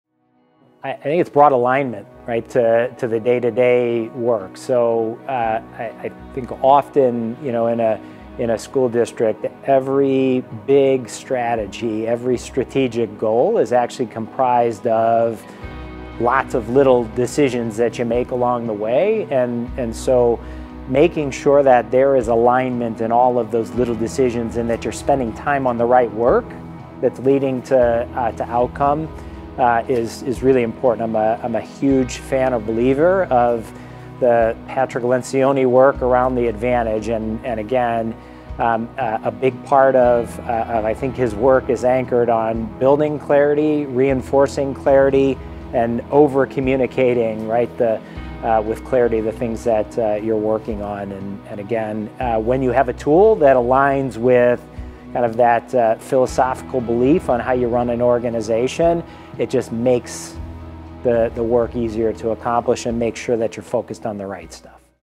Does a Strategic Dashboard Affect Day to Day Work?
Dr. Schuler, Superintendent of Schools at Wheaton 200, discusses how a Strategic Dashboard has the potential to impact day to day work at a school district.

What is a Strategic Dashboard?
A strategic dashboard is a powerful tool for superintendents and boards of education to tell their school district’s story and track strategic indicators and priorities.